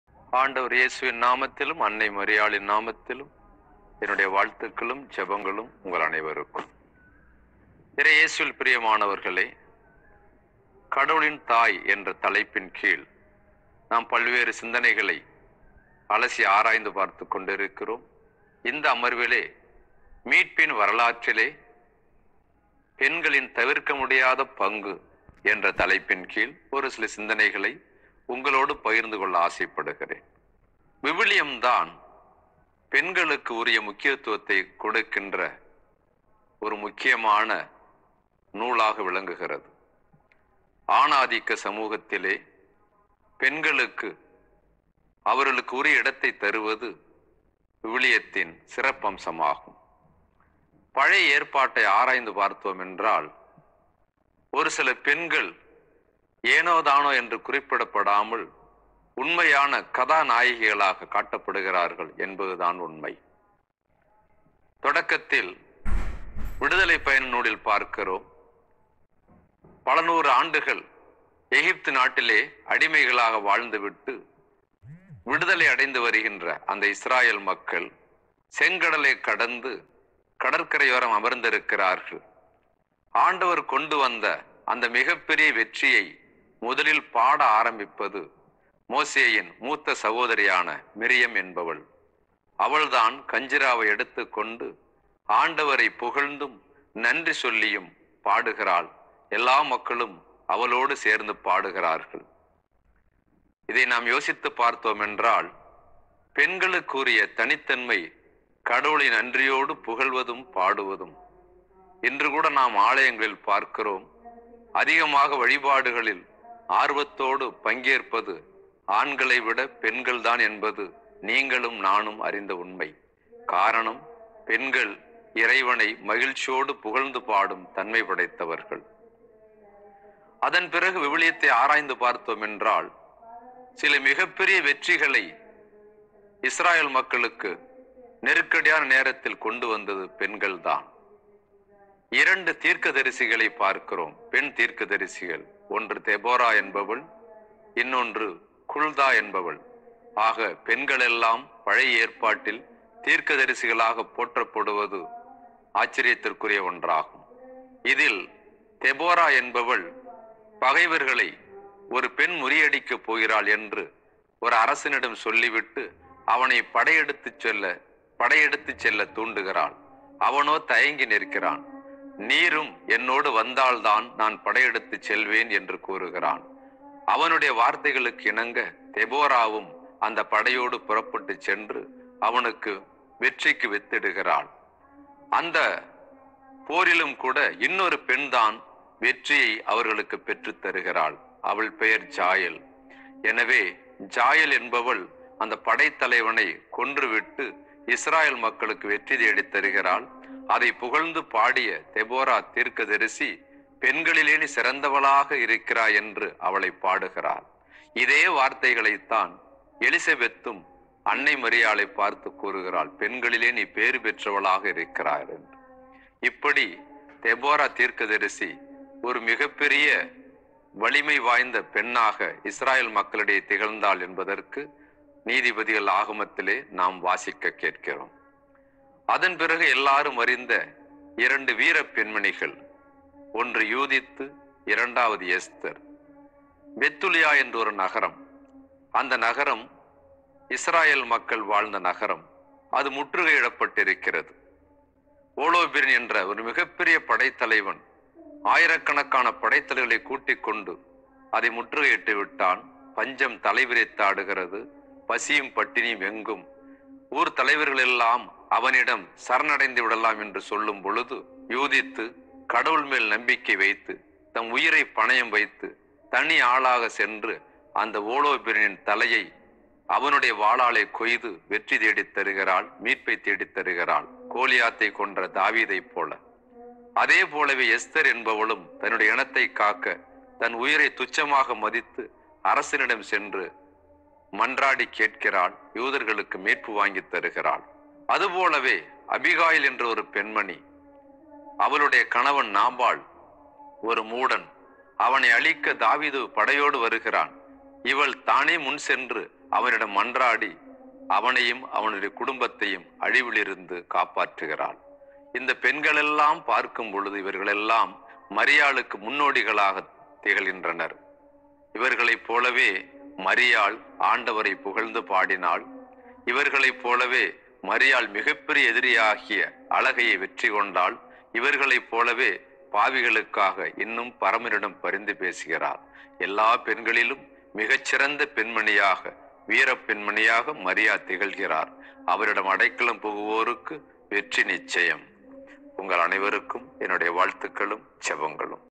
The Role of Women in Salvation History | மீட்பின் வரலாற்றில் பெண்களின் பங்கு
The Role of Women in Salvation History | மீட்பின் வரலாற்றில் பெண்களின் பங்கு @TamilCatholicChannel

A Short discourse on the brave women of Old Testament like Deborah, Esther, Judith and Jael, comparing them with Mary who is above all of them who brought decisive victory to mankind.